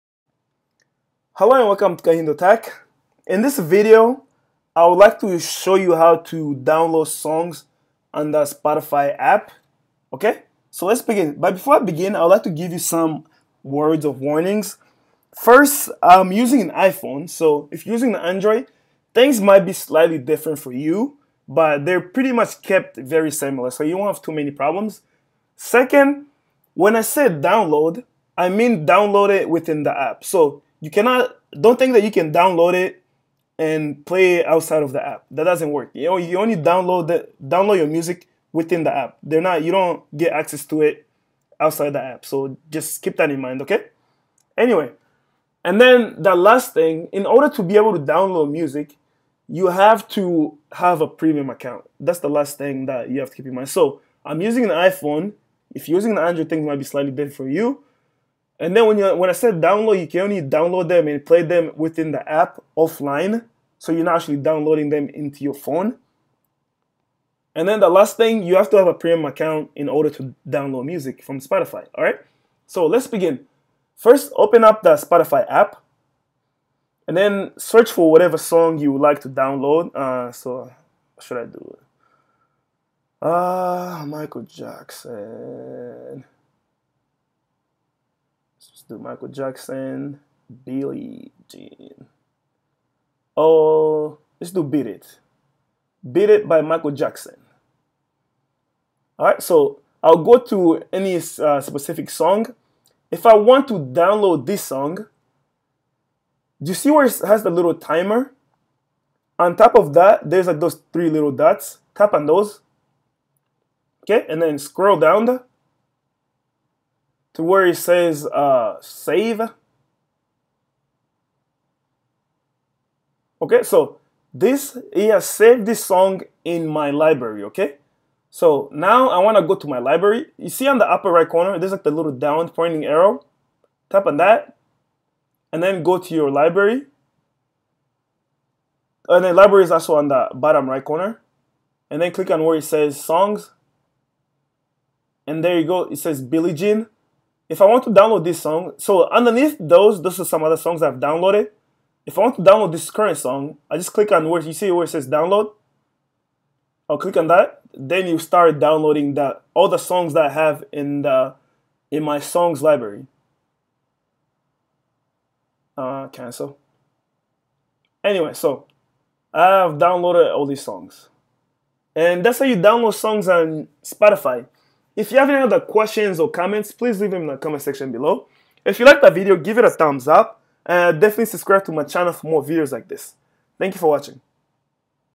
How To Download Songs Spotify iPhone
This video shows How To Download Songs Spotify.

►► FREE Amazon Prime Music 30-Day Trial